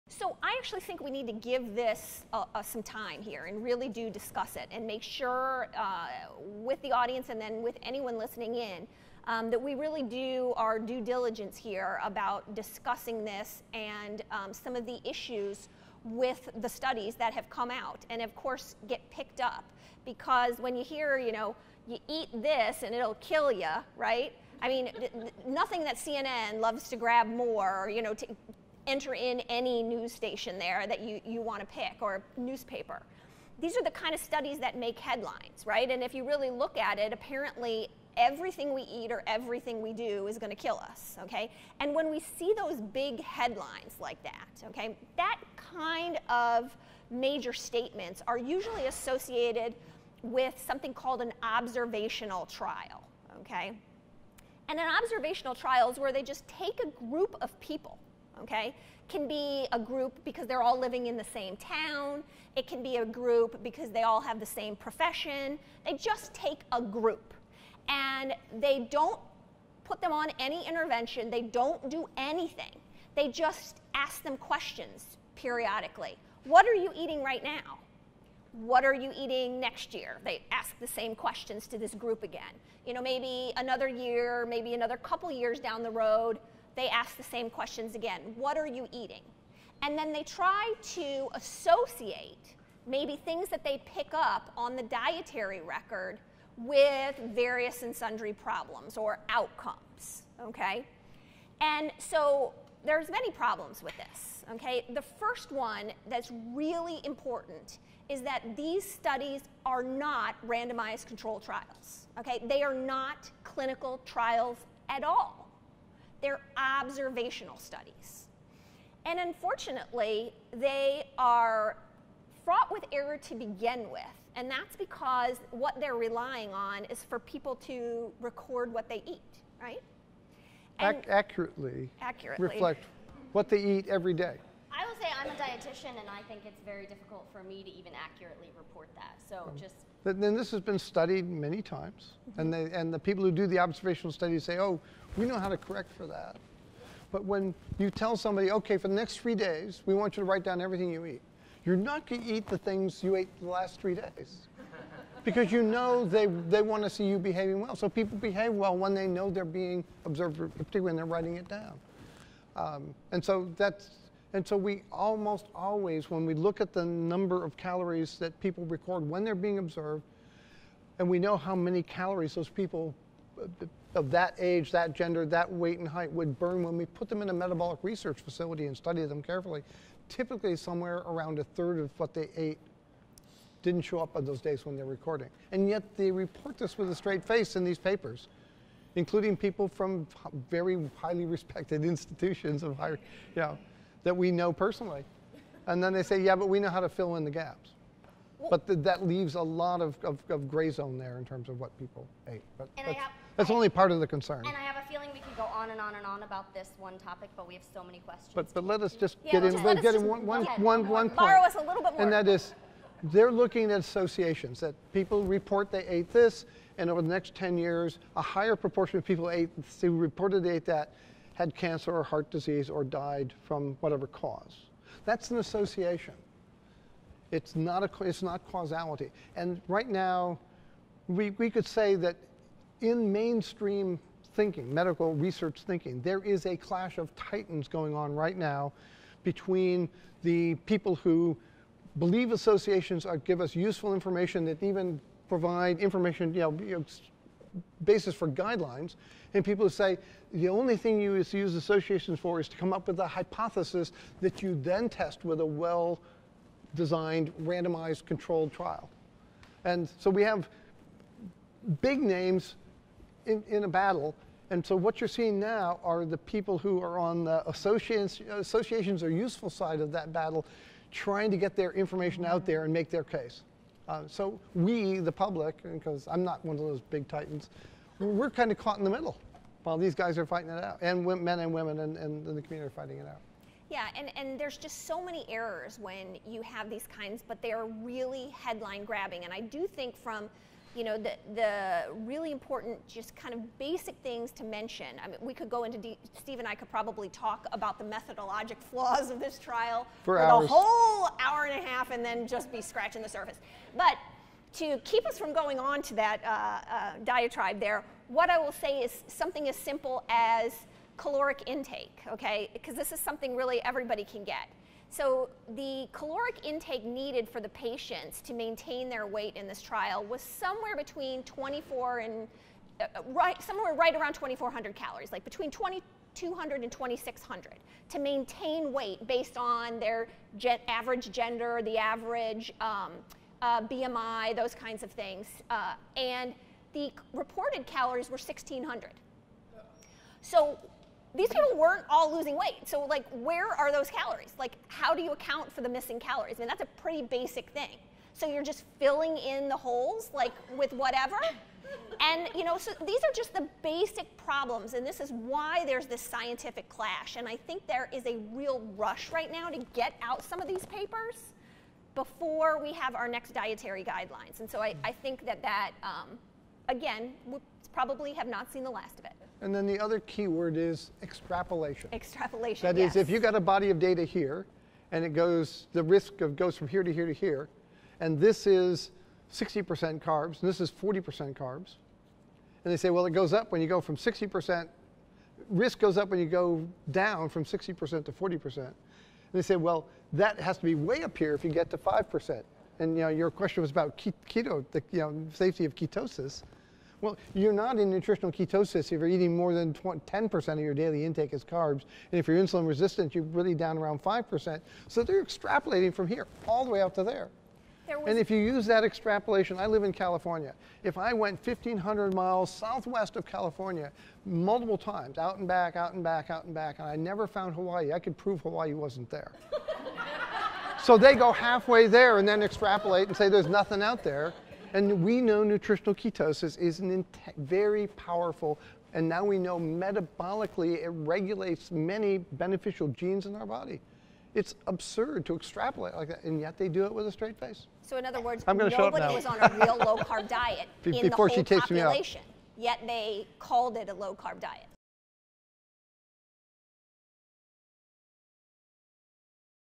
Dr. Phinney and Dr. Hallberg on the limitations of observational studies of ketogenic diets
Dr. Sarah Hallberg is a physician, board-certified lipidologist, Medical Director at Virta Health, Medical Director at the Indiana University Health Medical Weight Loss Program, and Executive Director at The Nutrition Coalition. She is the primary investigator on a recent clinical trial examining a technology-supported and medically-supervised ketogenic intervention that reverses type 2 diabetes and prediabetes.

Stephen Phinney, MD, Ph.D is the Chief Medical Officer and Co-Founder of Virta Health, the first clinically-proven treatment to safely and sustainably reverse type 2 diabetes without medications or surgery.